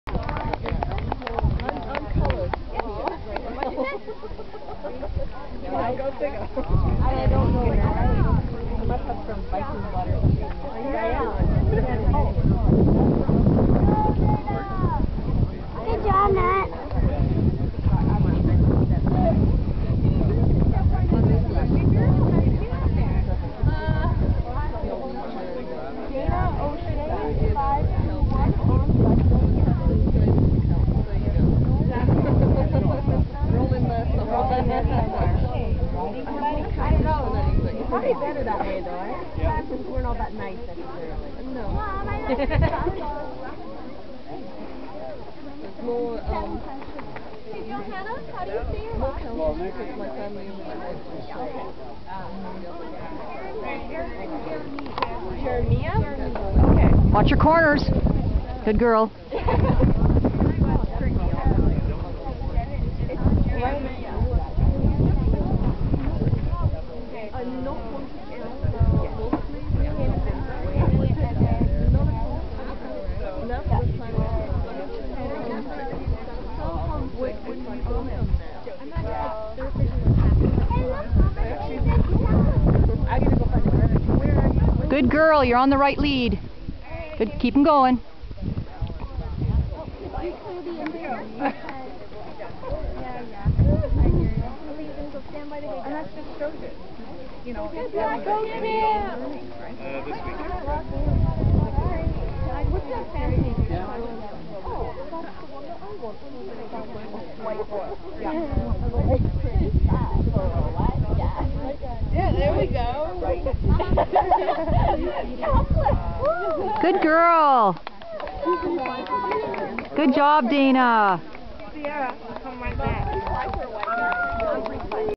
Dana and buddy Novice Show
Dana and Buddy my quarter horse reiner  doing a novice show when she was 8